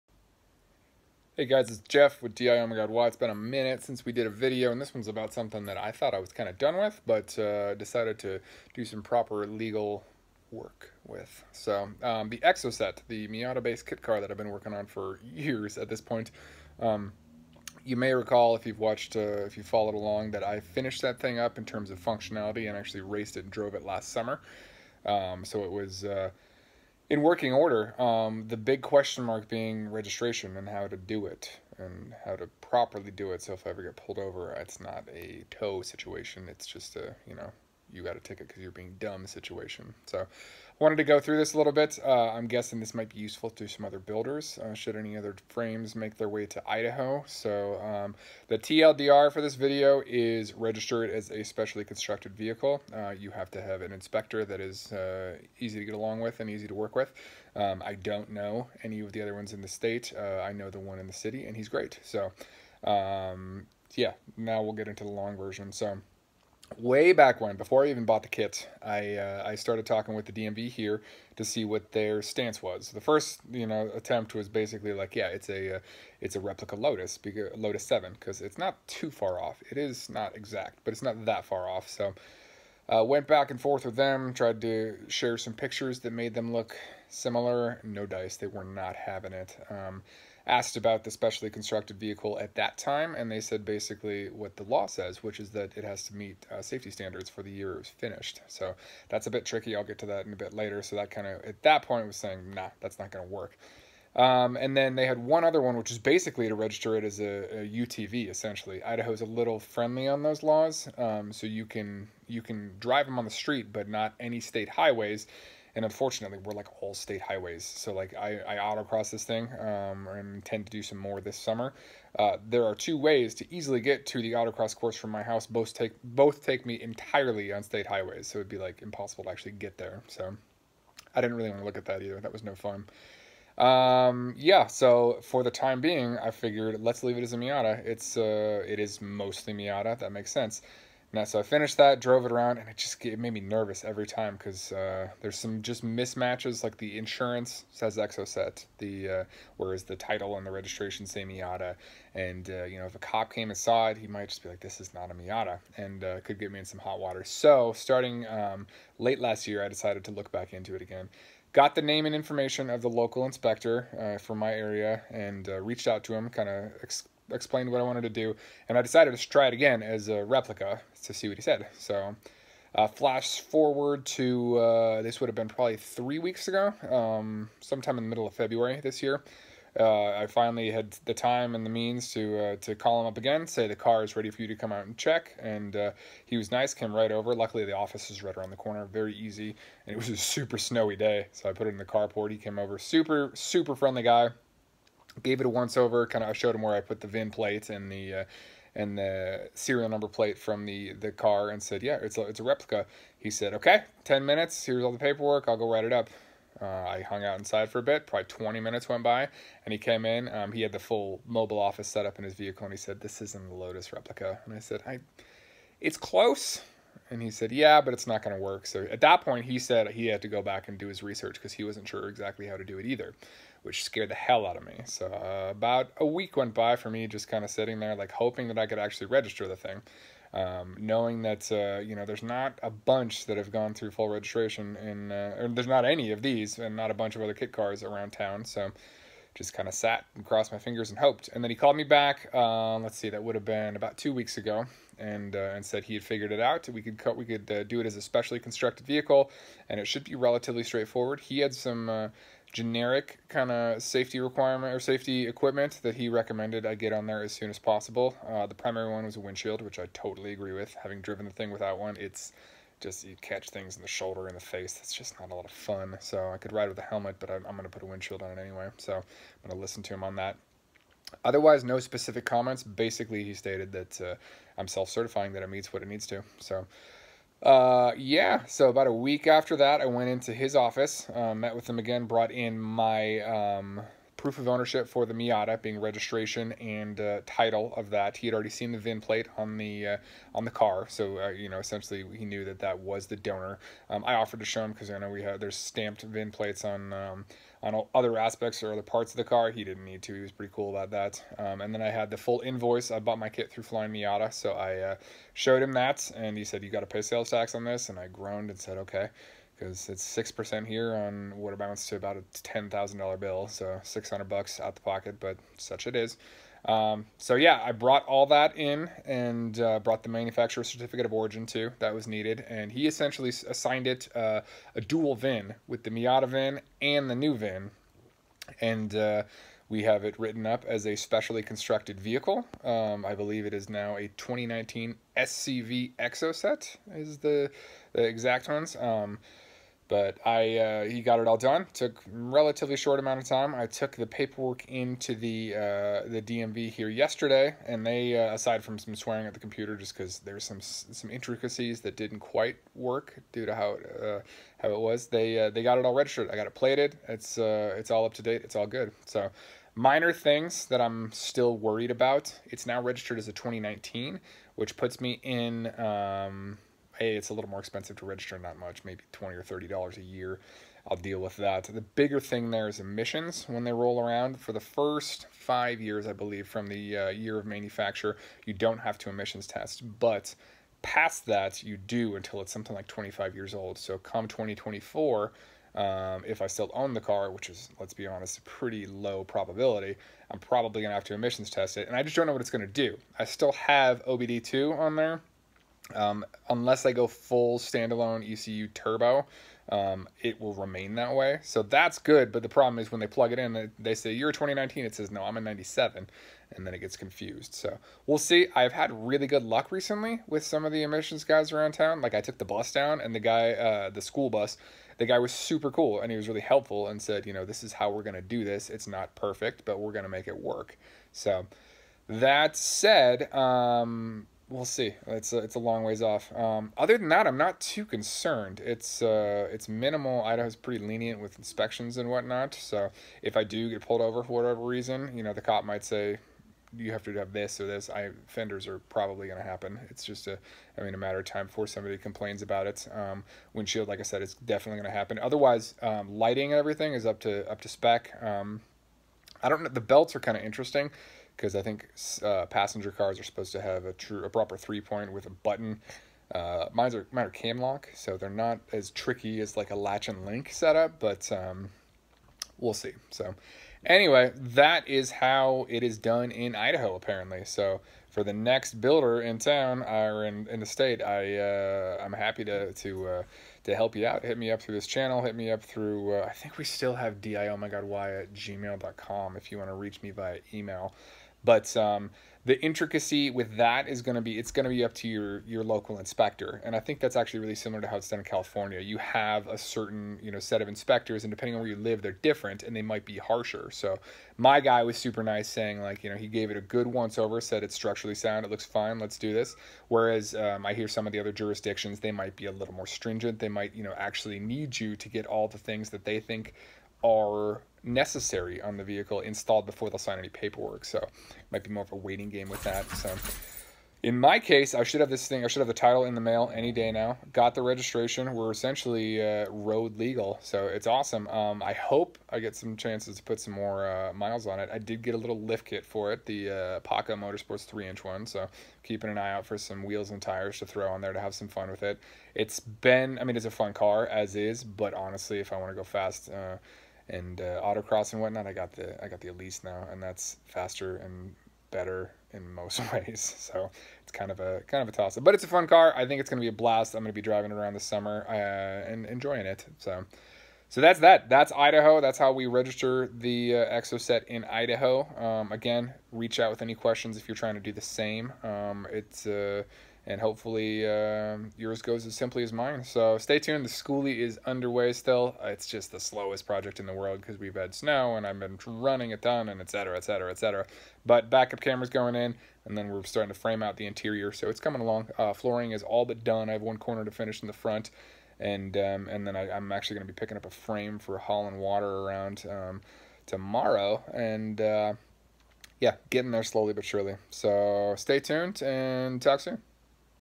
Exocet Registration in Idaho | DIomgY
Walking through the steps I had to take to get my Exomotive Exocet street legal in Idaho.